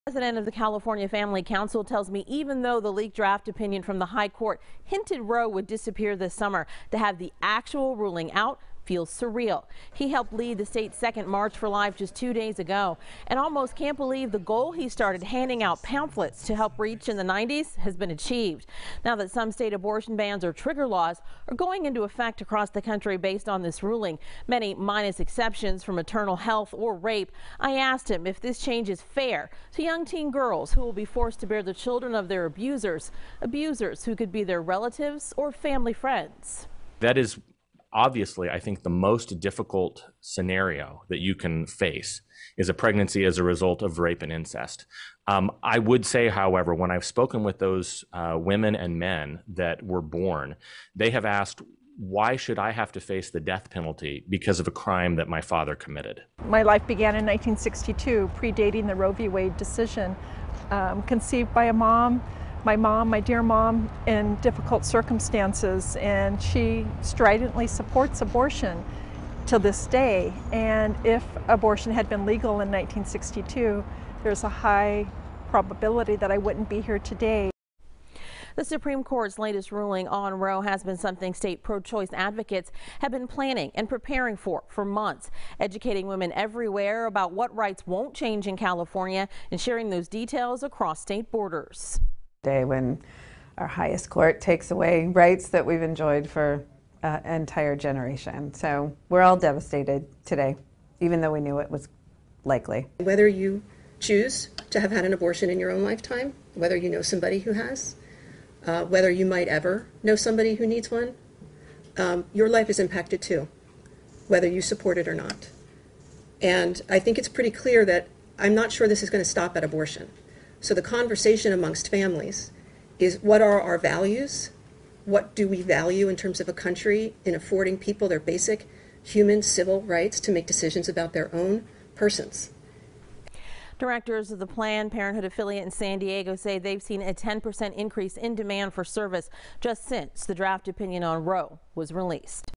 Sacramento-area advocates for and against abortion state their thoughts on the Supreme Court ruling
Anti-abortion advocates say that people whose parents decided to not have an abortion appreciate the opportunity to live their lives, while abortion advocates say that the ruling takes away rights that an entire generation has been accustomed to having and that everyone's life is impacted.